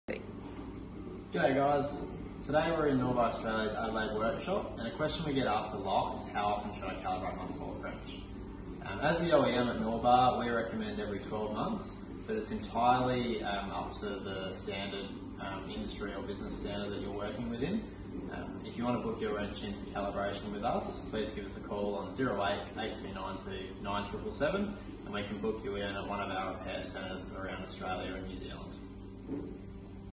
How Often Should I Calibrate My Torque Wrench?
Norbar Australia's Torque Bolting Specialist explains how often you should calibrate your Torque Wrench.